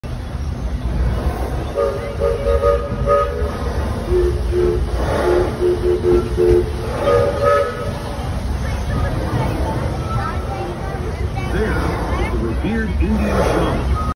Mark Twain and steam train have some fun!
Riding the Disneyland railroad as it passes the Mark Twain and they have some fun with their whistles!